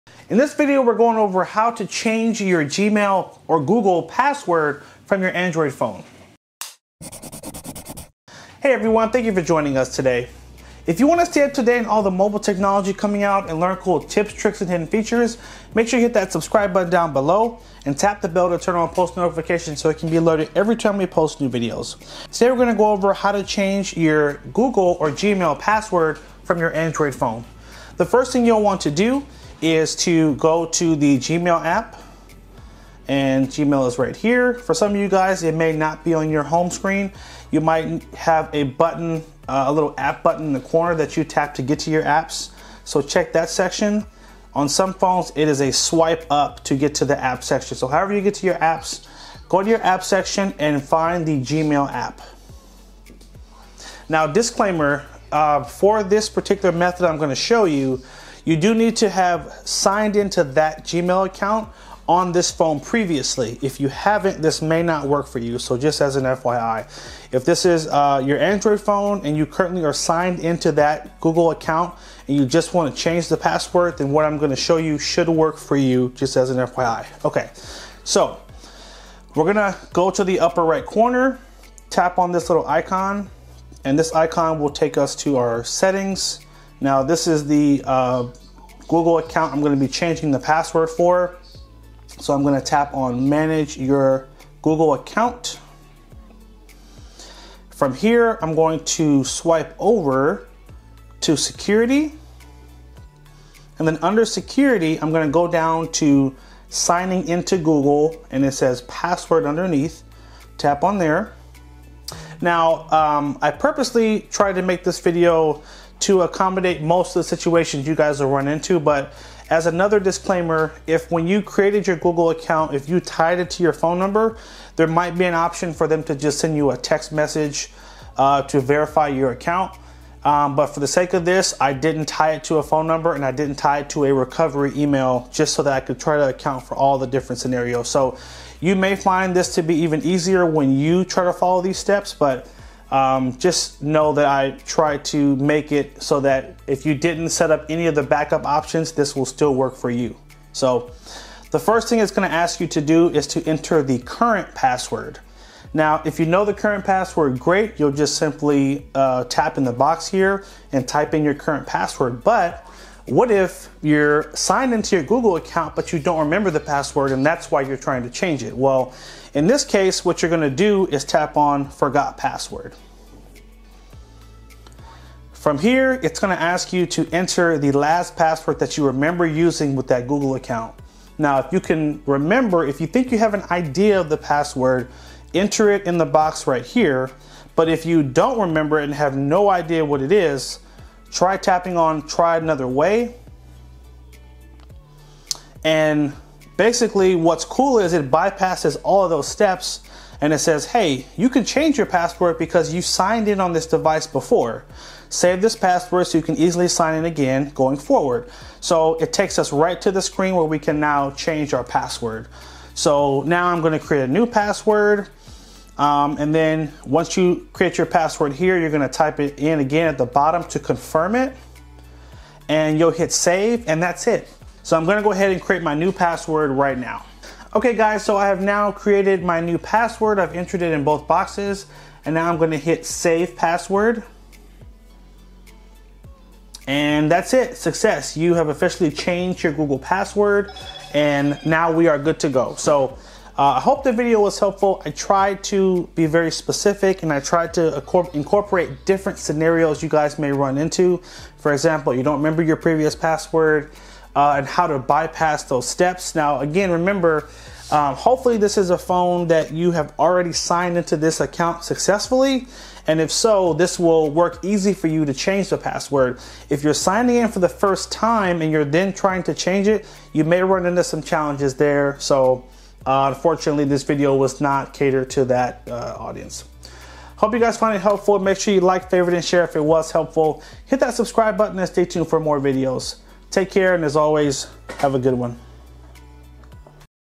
How to Change Your Gmail Password from a Phone | Google Password Reset
Step by step walkthrough on how to change your google gmail password on an android phone (Samsung, LG, OnePlus, Motorola, Pixel, etc)